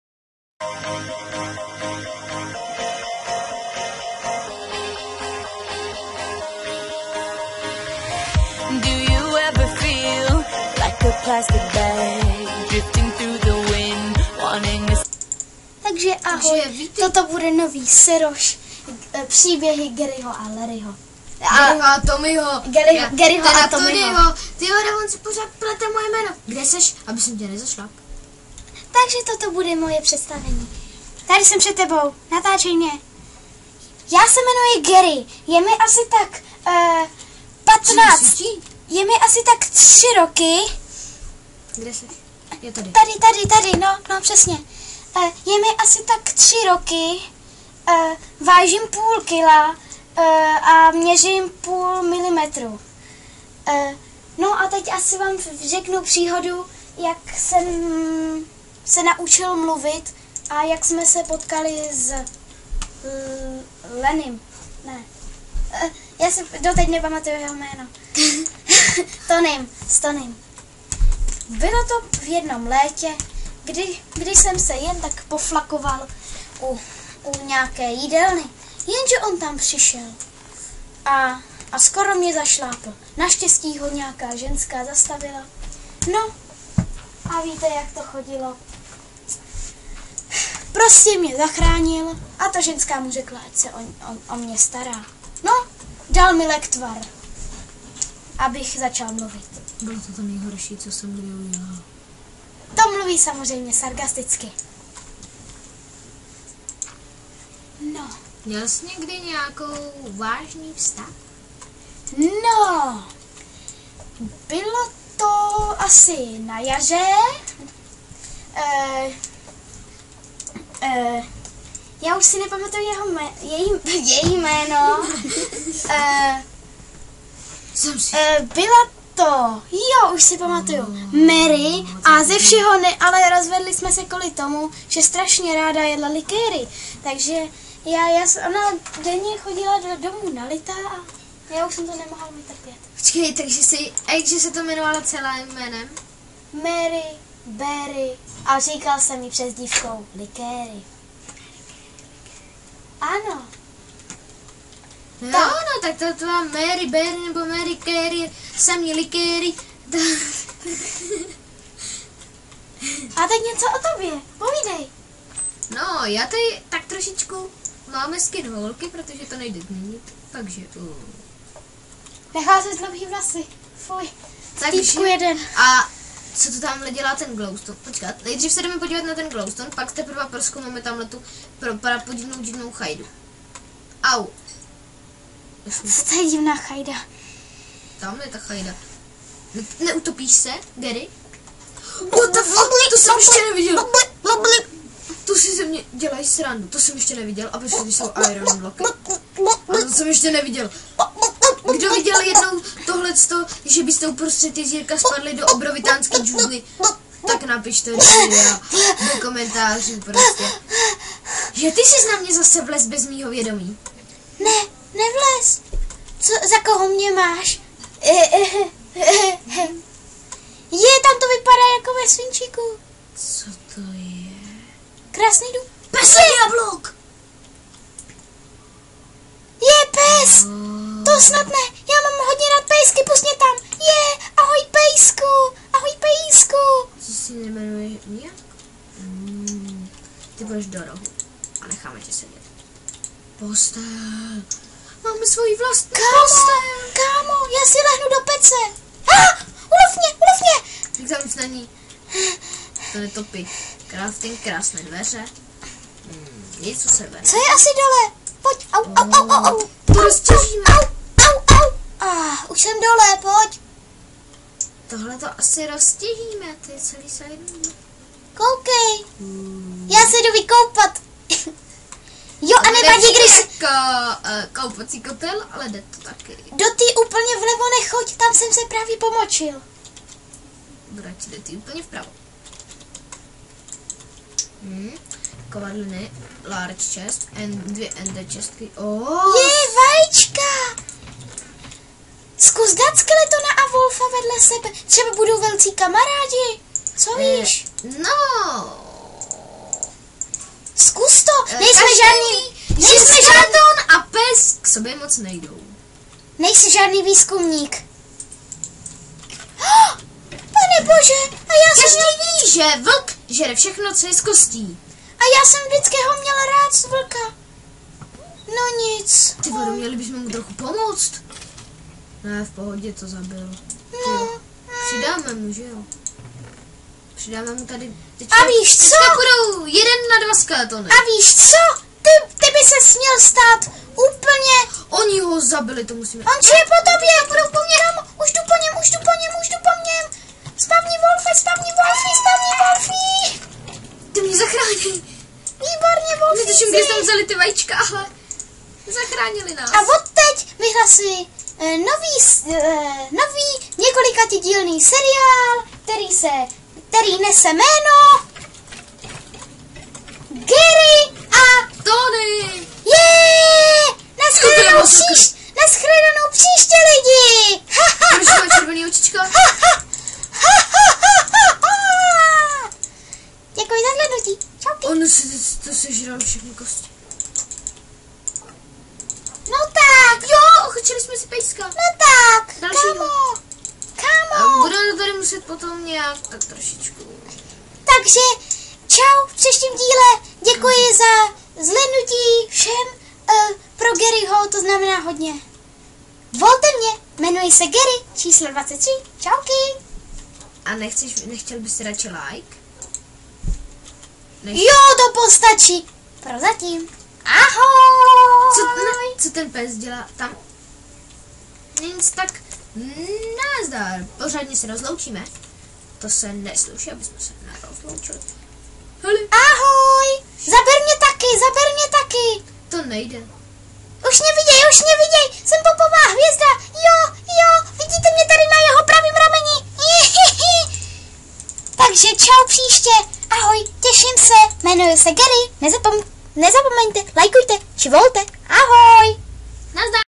příběhy garyho a tonyho EP.1
Gary a Tony oběví tajnou výzkumnou stanici